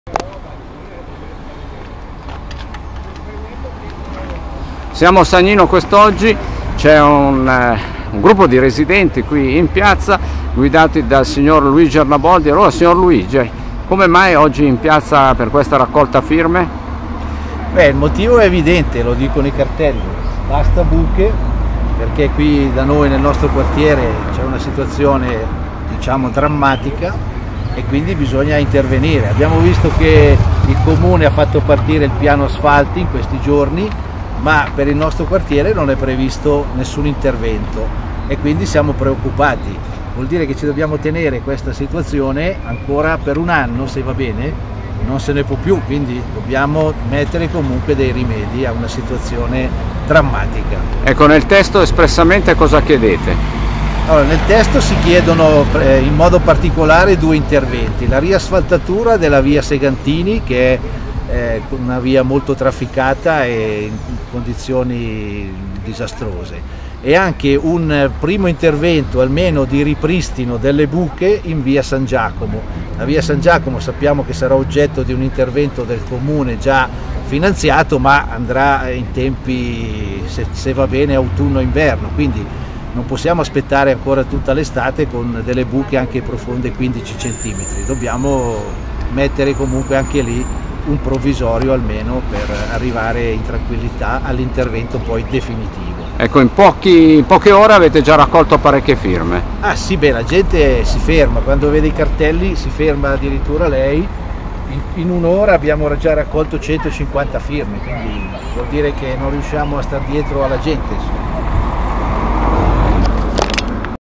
TANTE FIRME A SAGNINO PER CHIUDERE LE BUCHE
Parla un portavoce dei residenti: i loro disagi ora sulla scrivania del sindaco Lucini.